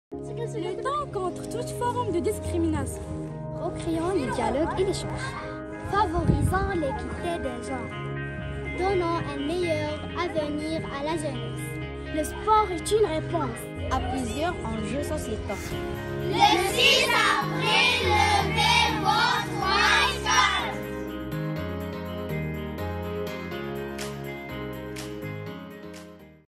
April6 challenge - Tibu Africa
Our partner of the Peacemakers Project Tibu Africa is getting mobilized for April6 by raising their WhiteCard!